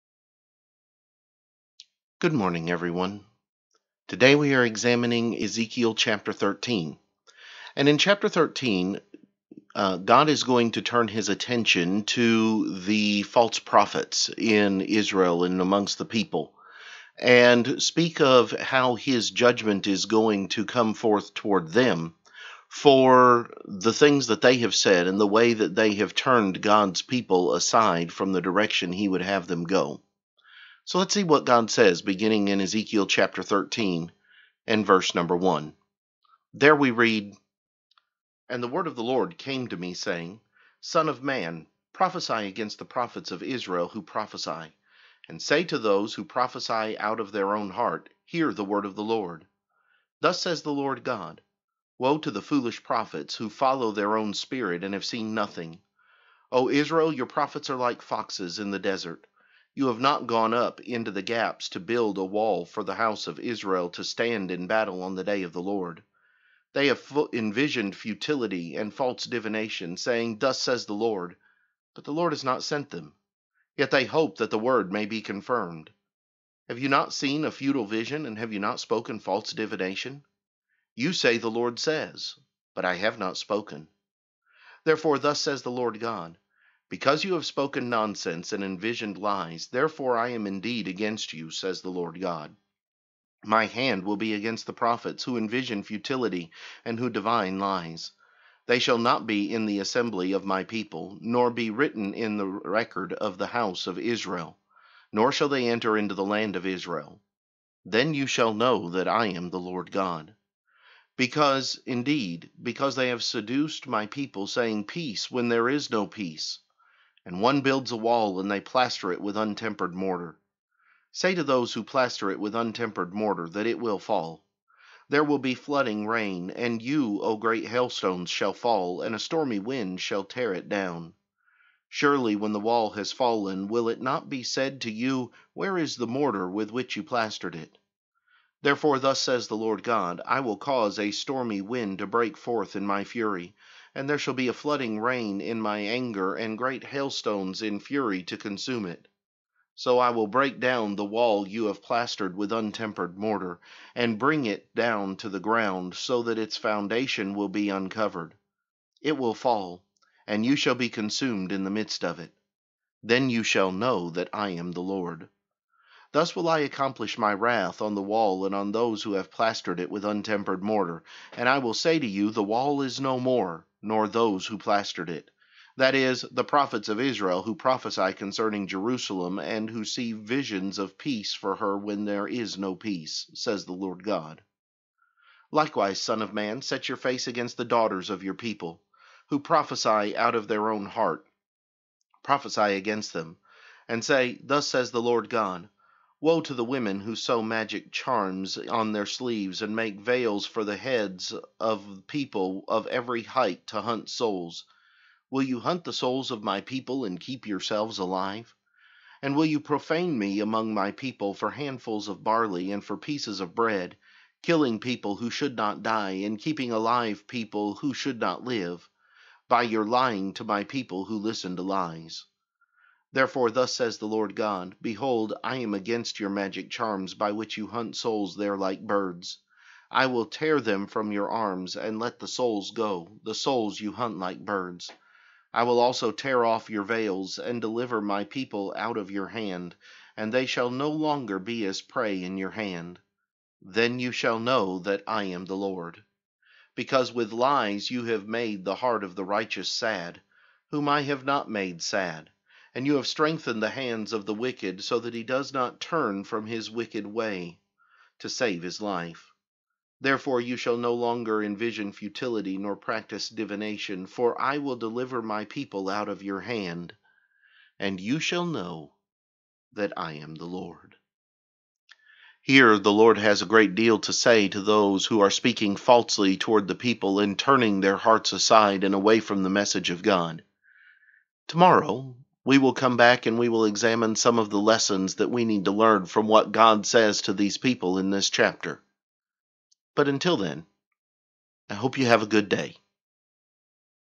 Ezekiel 13
Introducing and reading Ezekiel chapter 13.